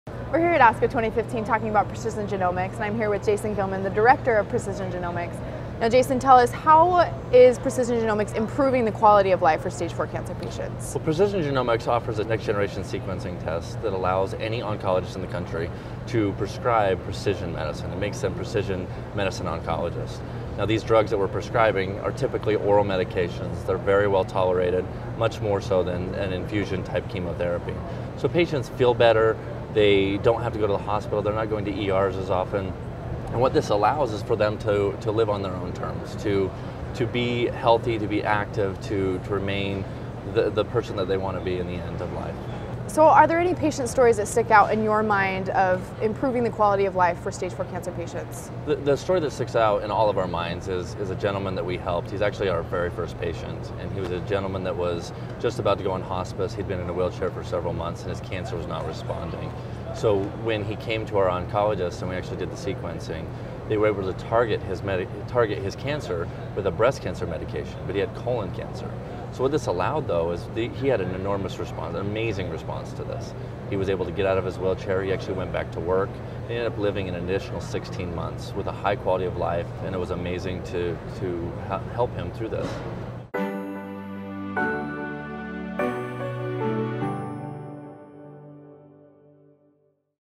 Improving Quality of Life for Stage Four Cancer Patients
Jason L. Gillman, Director of Precision Genomics at Intermountain Healthcare talks about improving quality of life for stage four cancer patients.